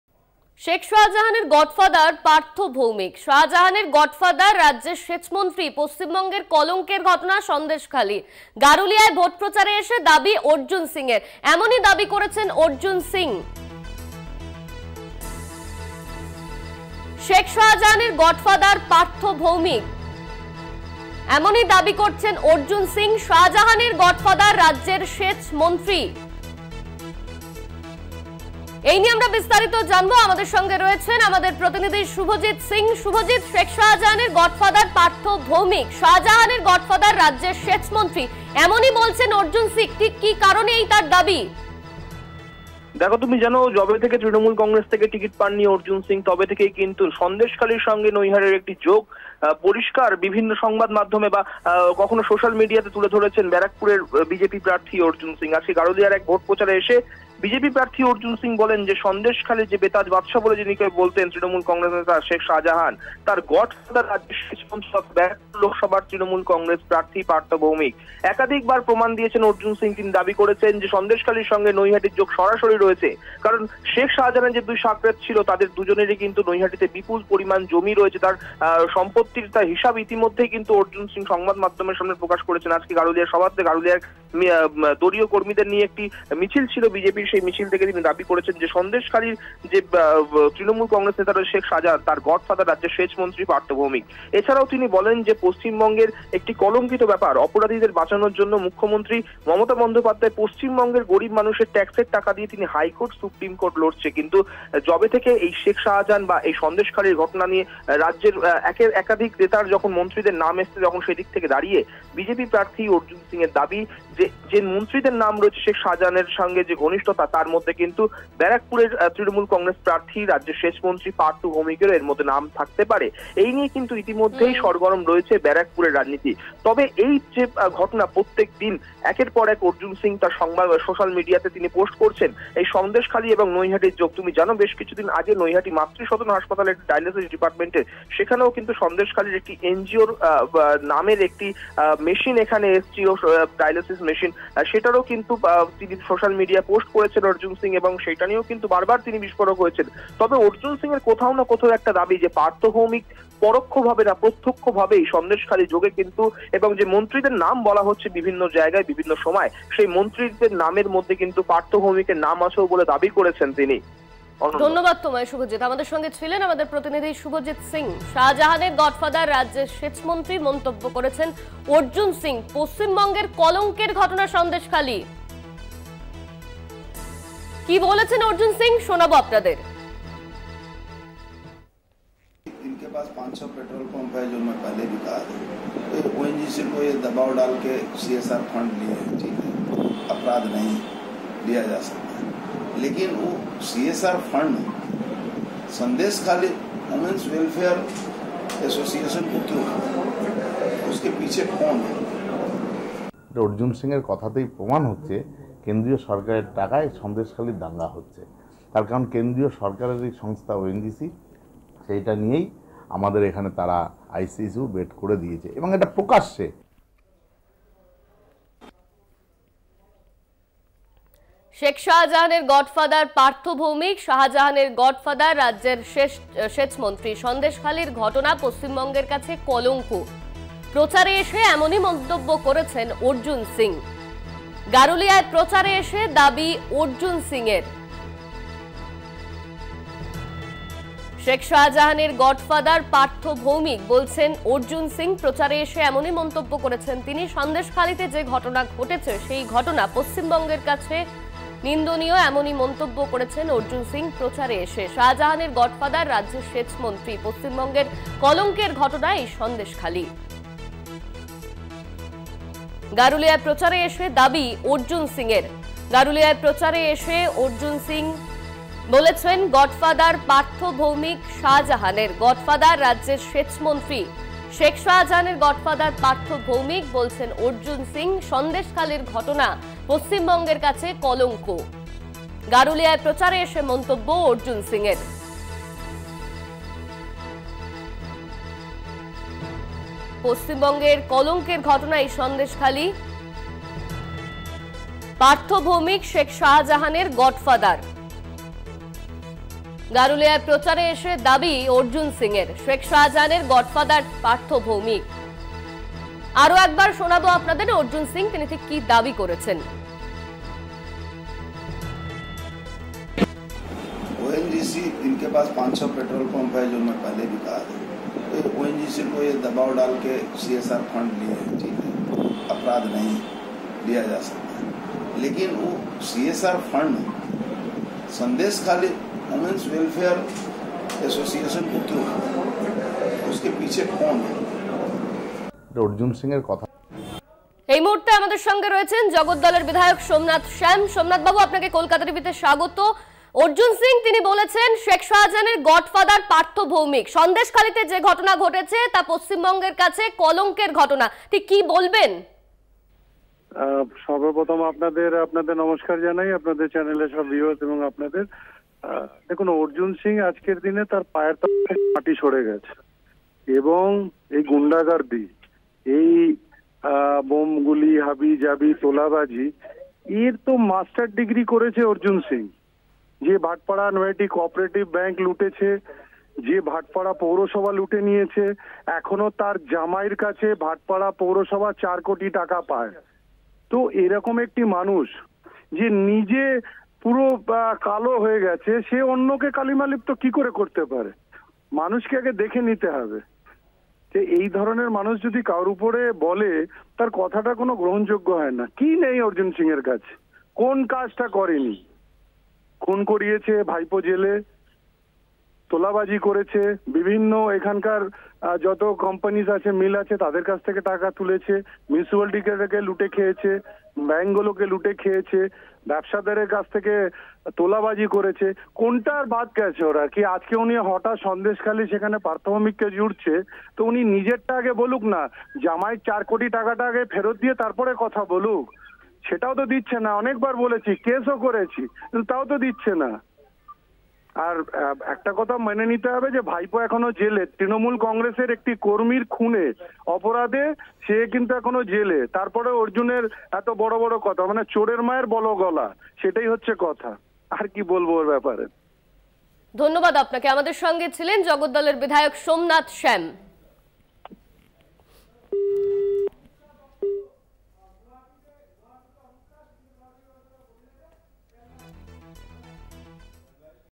Arjun Singh | 'শেখ শাহজাহানের গডফাদার পার্থ ভৌমিক', গারুলিয়ায় প্রচারে এসে দাবি অর্জুন সিংয়ের
website link for this copy :
@Kolkata Tv